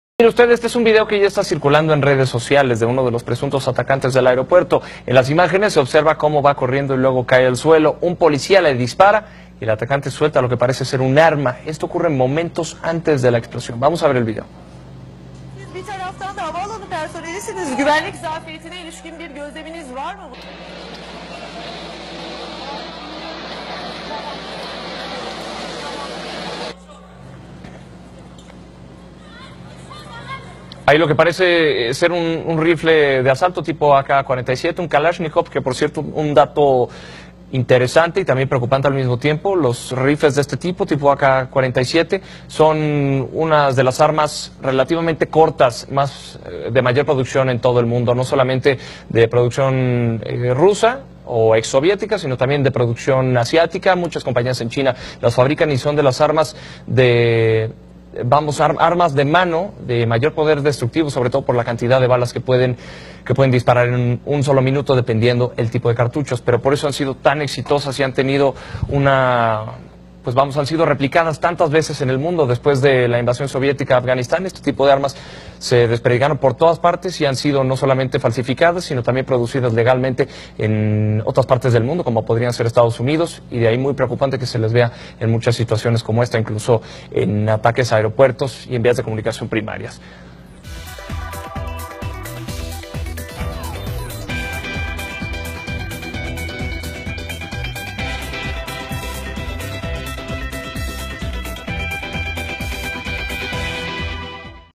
Presunto autor del atentado en aeropuerto de Estambul difundido en redes sociales:
Archivo 2016
Un ataque suicida en Turquía deja 41 muertos y mas de 239 heridos; entre las victimas locales figuran dos policías y trece extranjeros.